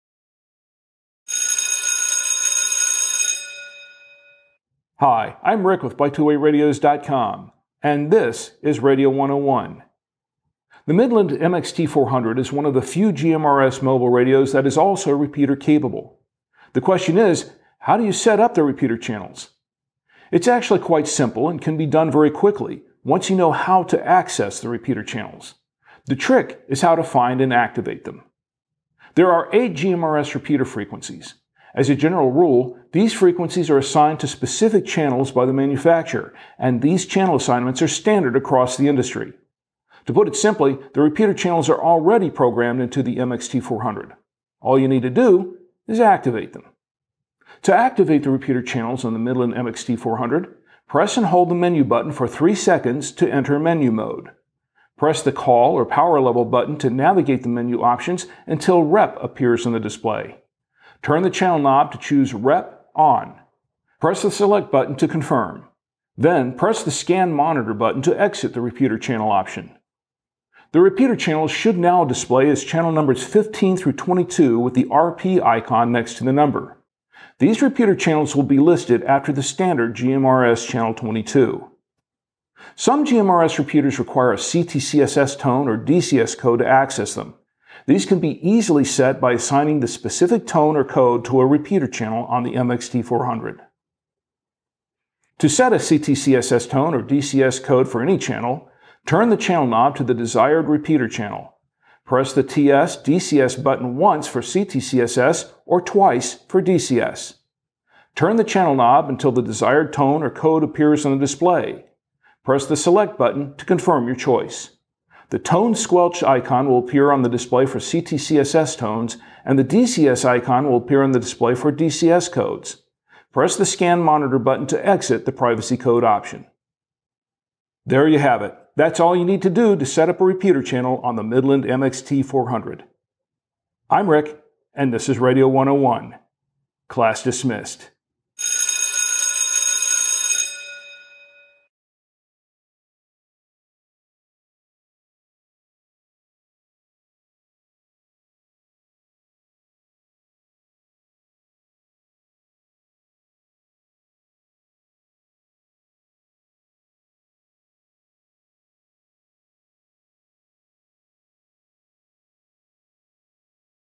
How to Set Repeater Channels on the Midland MXT400​ | Radio 101
Midland MXT400 GMRS mobile radio is repeater capable. The question is, how do you activate and set up the repeater channels? In this episode of Radio 101, we show you.